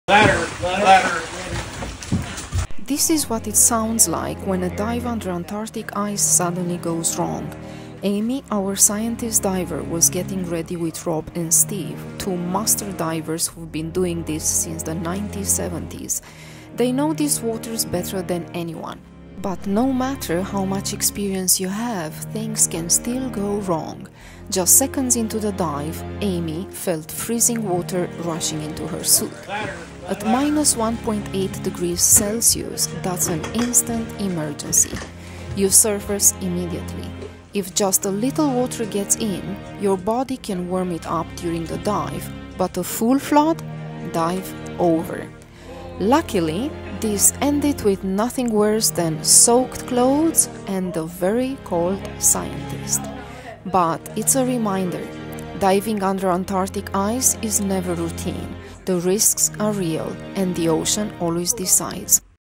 When an Antarctic dive goes wrong
‼️When a dive goes wrong 😬

Diving in Antarctica is one of the most extreme jobs in science. Even with decades of experience, things can go wrong. Amy, one of our scientist divers, had a zipper malfunction, cold water flooded her dry suit just seconds into the dive. At -1.8°C, that’s an instant emergency ascent.

This is why Antarctic diving is so risky: the water under the sea ice is colder than freezing, and every small mistake can escalate quickly. Luckily, this one ended safely! Just a soaked suit and a very cold scientist.

🇷🇴 Scufundările în Antarctica sunt printre cele mai extreme misiuni științifice. Chiar și cu zeci de ani de experiență, pot apărea probleme.

Amy, una dintre scafandrele noastre, a avut un fermoar defect la costumul uscat, iar apa rece i-a inundat costumul în doar câteva secunde. La -1,8°C, asta înseamnă urcare de urgență imediată.

De aceea scufundările în Antarctica sunt atât de riscante: apa de sub ghețar este mai rece decât punctul de îngheț, și orice mică greșeală poate deveni periculoasă. Din fericire, totul s-a terminat cu bine 😊doar un costum ud și o cercetătoare foarte înghețată.